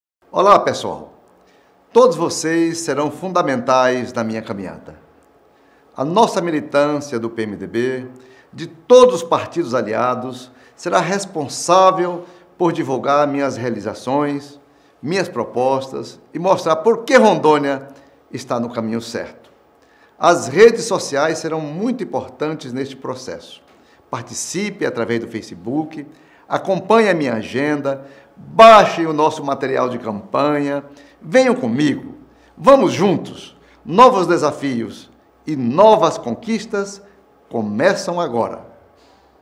Confúcio Moura convoca a militância
Confúcio Moura mobiliza militância para participar de sua campanha na internet.

Veja como você pode ajudar e participar em nosso site oficial, através do